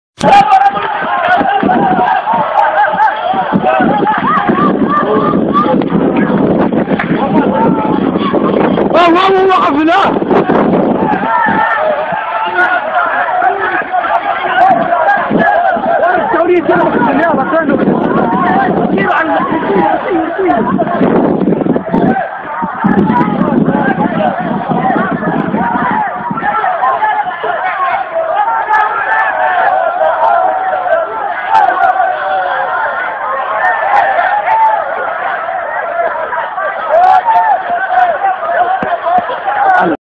هجوم أنصار النادي القنيطري على جماهير المغرب التطواني
خطير: هجوم أنصار النادي القنيطري على جماهير المغرب التطواني.. (فيديو حصري)